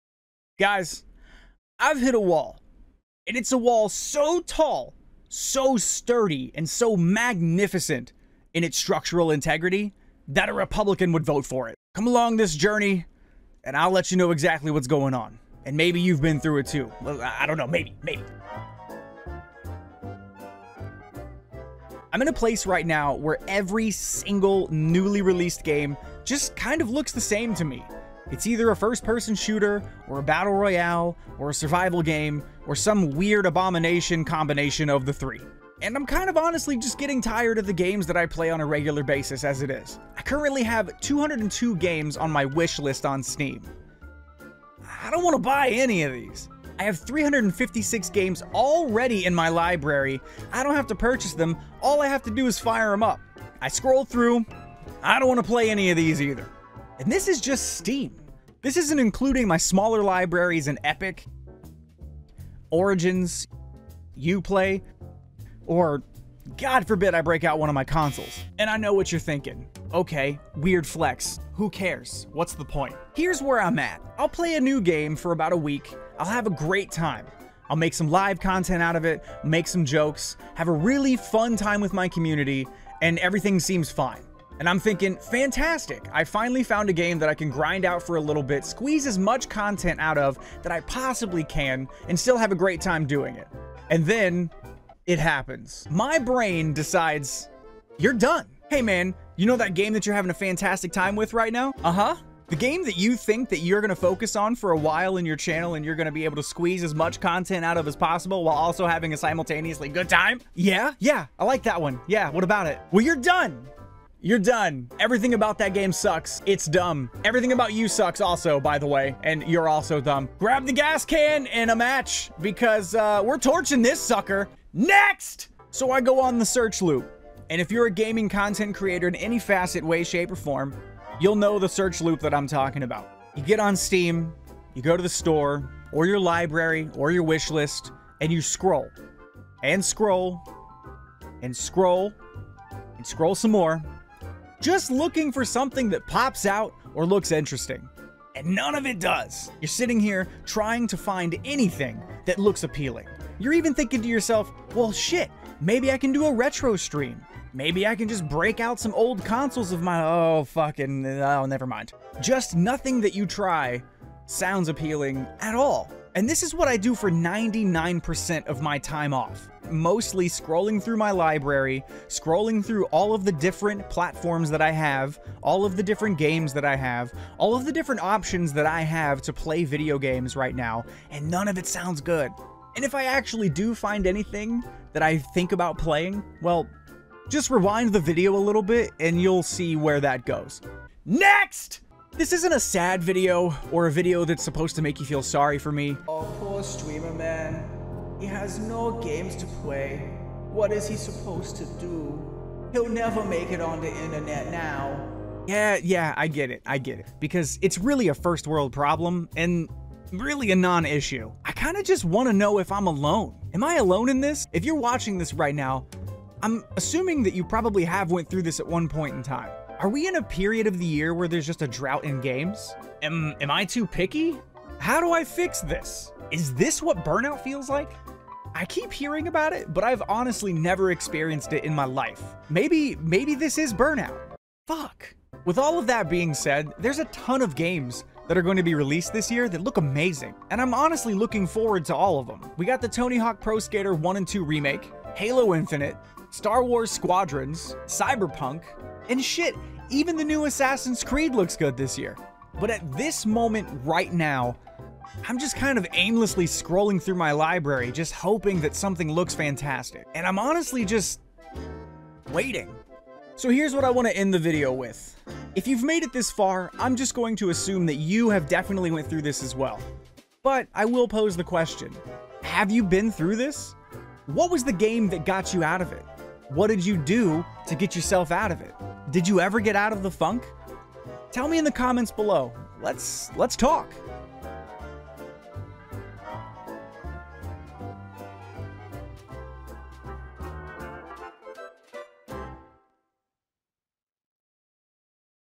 Gaming Burn Out. Is THIS What It Feels Like?
Am I currently having a Video Game "Burnout"? I explain how I'm feeling recently, and let you be the judge. Have any of you guys ever been through this? If so, What did you do to fix it?